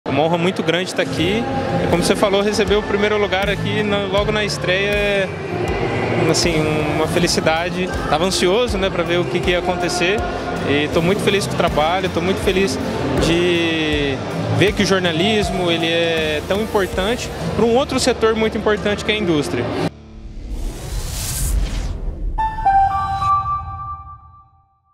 9º Premio Sistema Fiep de Jornalismo | Breno Ortega
Na categoria Fotojornalismo, um estreante no prêmio, Breno Thome Ortega, ficou na primeira posição com uma produção publicada no portal O Maringá, que reforçou a sustentabilidade no setor madeireiro.